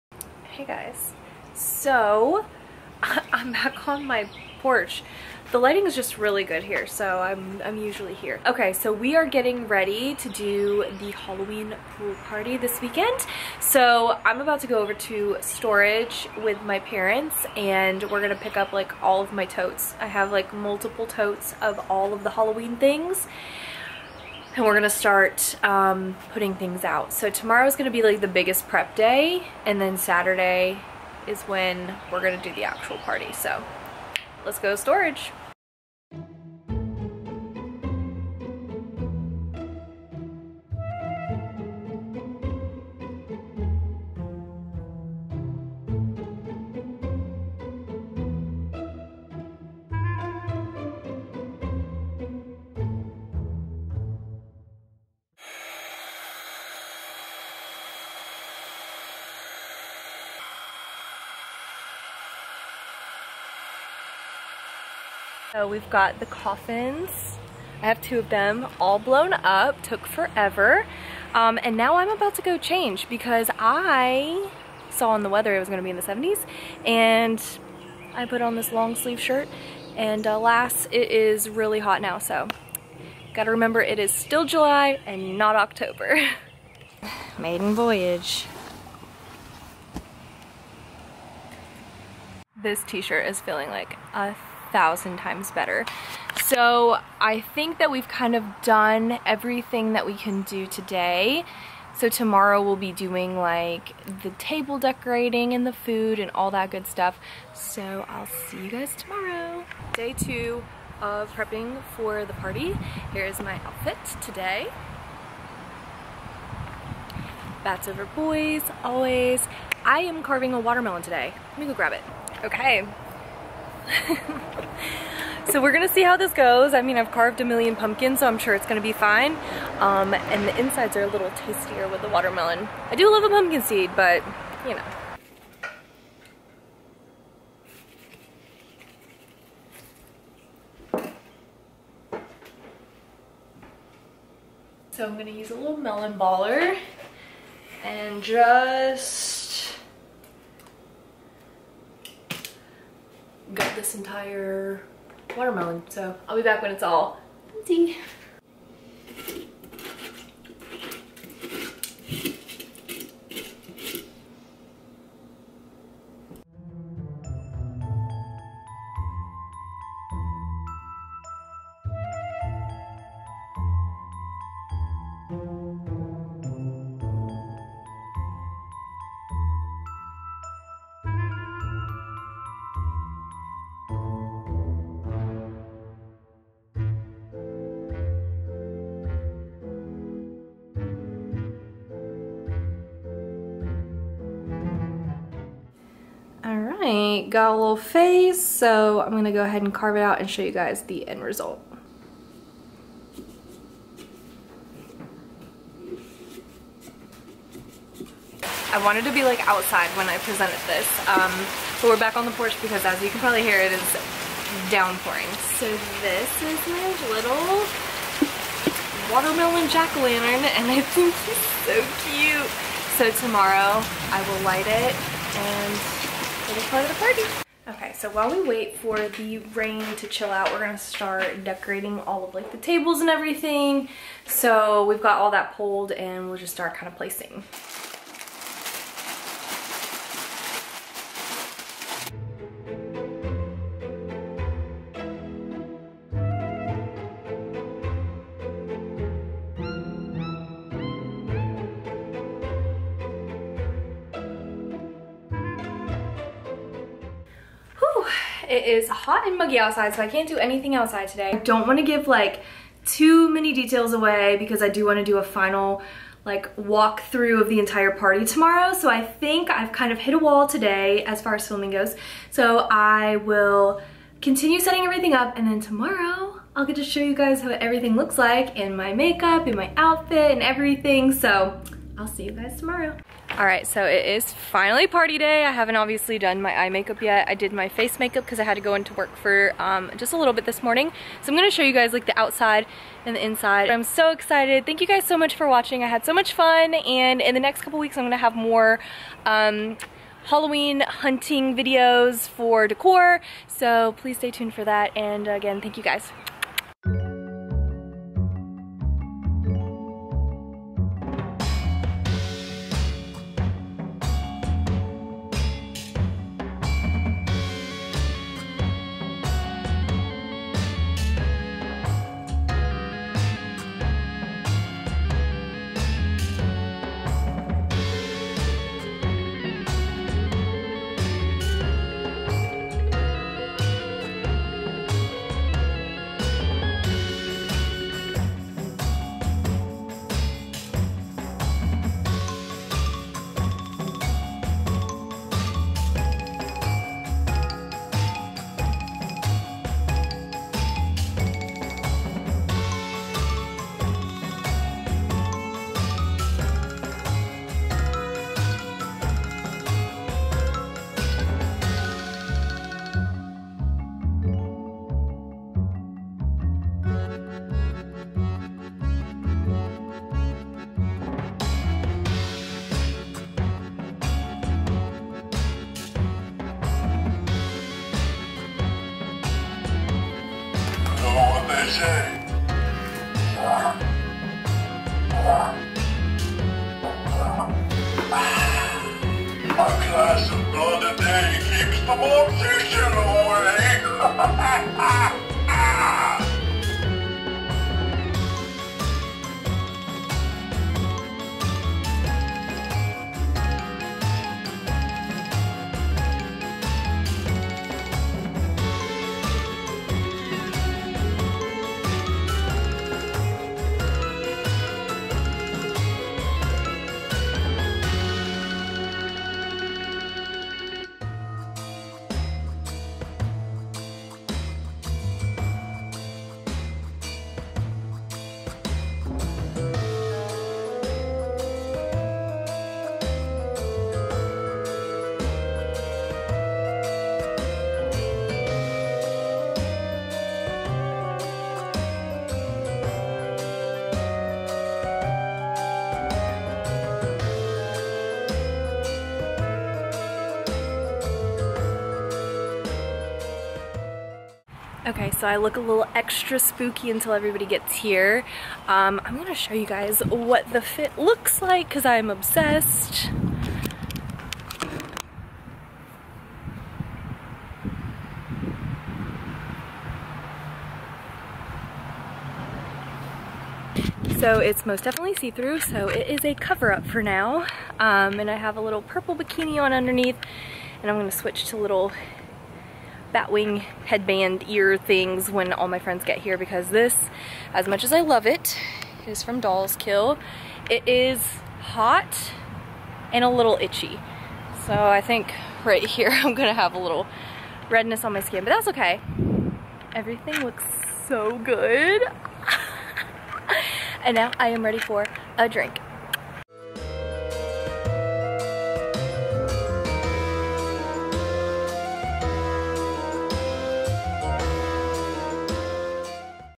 Summerween 2021 Vlog | Pool Party With Halloween Decor
Hello hello! In today's video I'll be showing you how I prepped to have a Summerween pool party for friends and family. I took way long than it should to get this one up, but I've been so consumed lately with Halloween hunting, but here it is finally!! Let me know what you did if you celebrated Summerween this year! xo